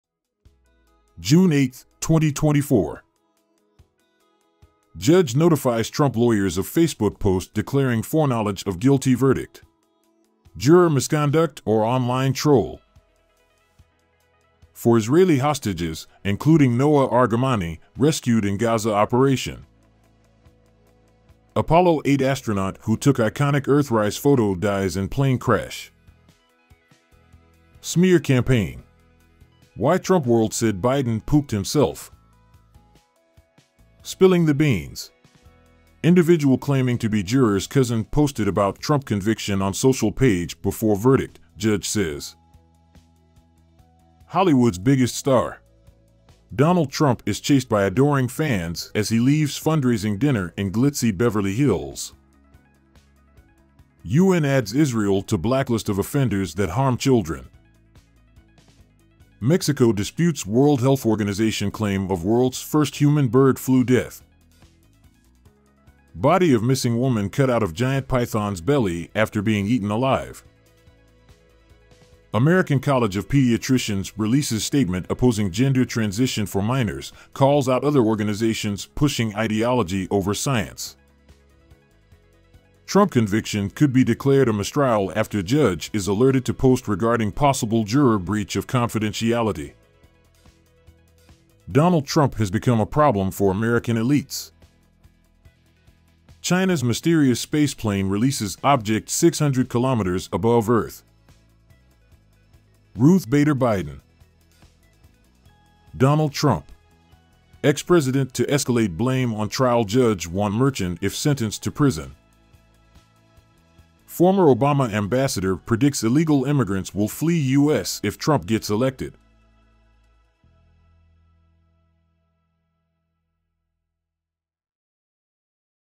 Headlines - June 8, 2024
A Proper Gander at News. ⭐️⭐️⭐️⭐️⭐️

A headline from a handful of news sources on the given date. Featured on the website ministryoftrutharchive dot com

The AI art is generated using the headlines and a set of terms.
Audio is also AI generated.